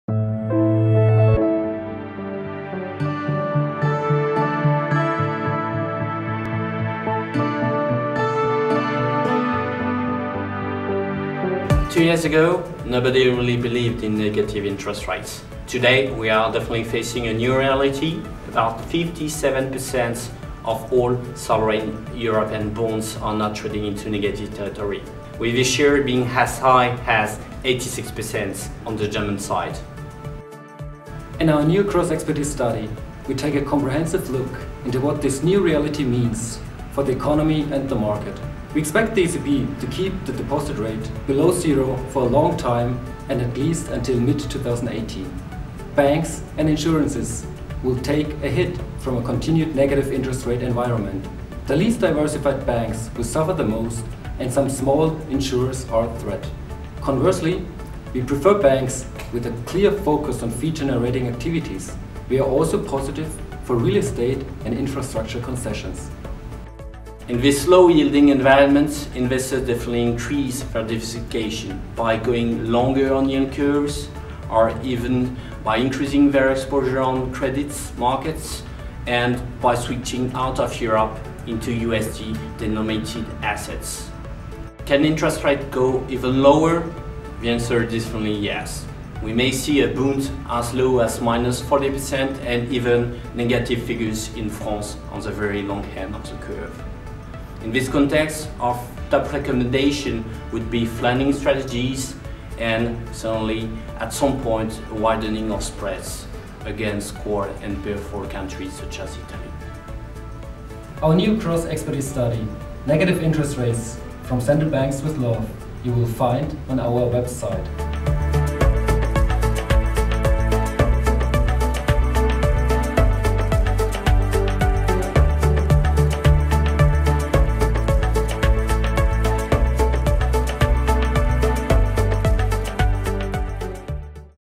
Les Rendez-vous Research by Natixis (ep. 9)
« Taux négatifs – Bons baisers des banques centrales  »

La recherche cross-expertise de Natixis a présenté le 7 juillet dernier sa dernière étude s’intitulant «Taux négatifs – Bons baisers des banques centrales  ».

portail d’accès unique à la recherche cross-expertise.

« Negative interest rates – from central banks with love »

On July 7, Natixis’ cross-expertise research team presented its new study called “Negative interest rates – from central banks with love”

the single cross-expertise research website.